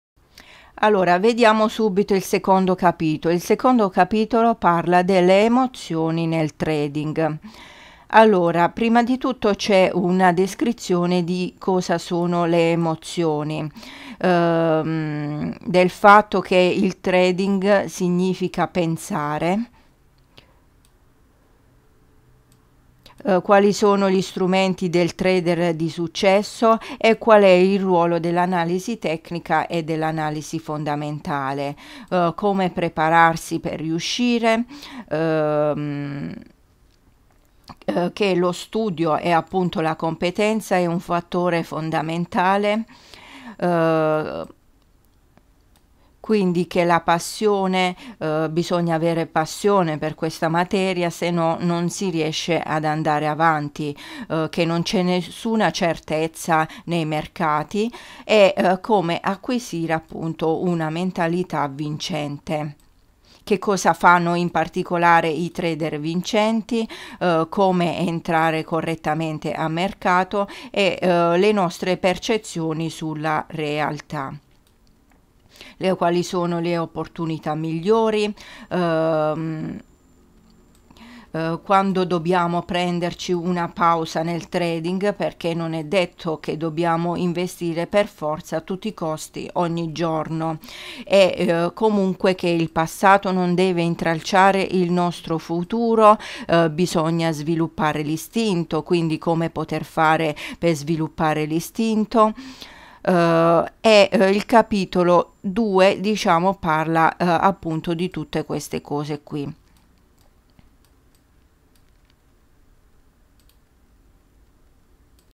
opzioni binarie, forex: La psicologia del trader capitolo 2
Quest'ebook parla della psicologia del trader, una parte molto trascurata da tutti i trader, ma che compone circa l'80% del successo nel trading. Nell'ebook è spiegato qual'è il comportamento che il trader deve adottare per essere vincente e vengono proposti una serie di esercizi da mettere in pratica per allenare la mente al successo.